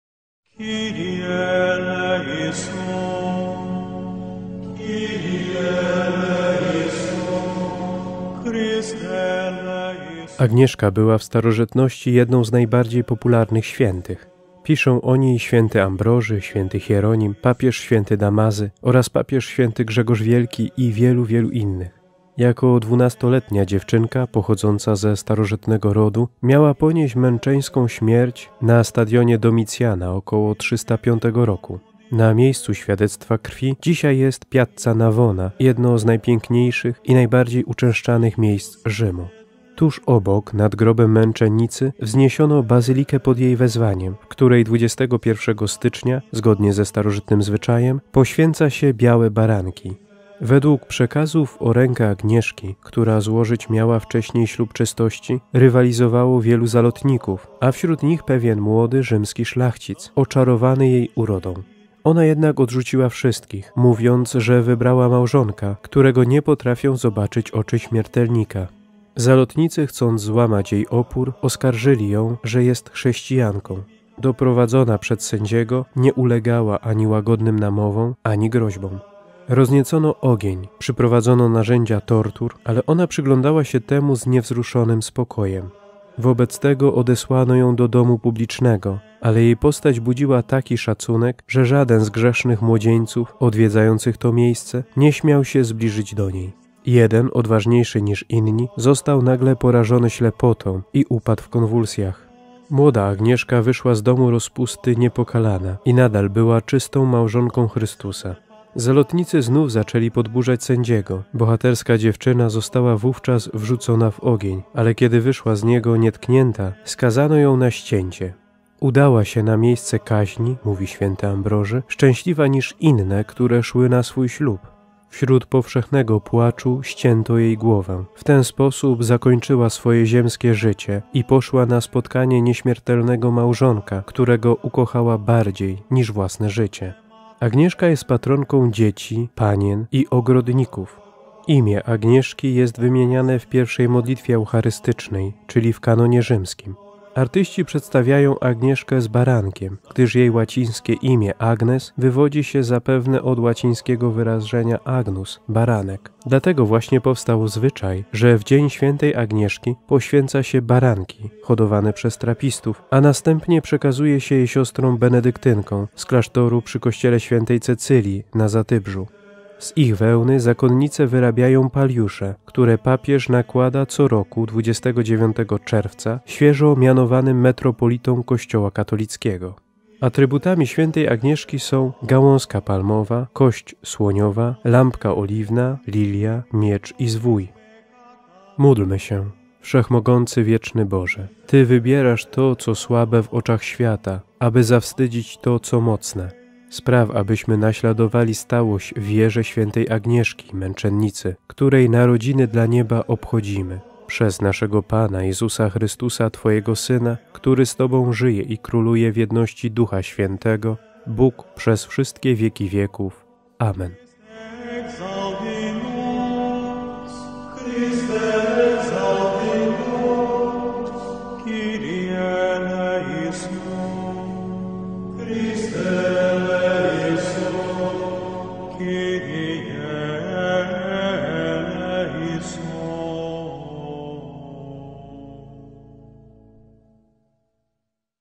Skarby Kościoła 21 stycznia | św. Agnieszka
Dnia 21 stycznia wspominamy św. Agnieszkę, męczennicę z Rzymu.